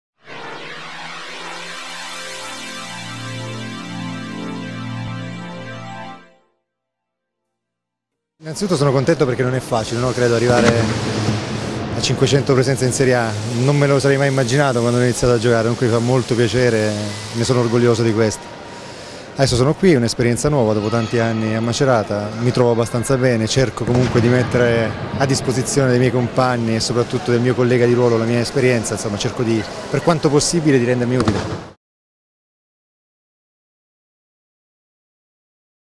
A1 Sustenum - Mirko Corsano 500 gare in serie A
Il libero della M.Roma domenica, prima della gara, è stato premiato dal Ct azzurro Mauro Berruto per il prestigioso traguardo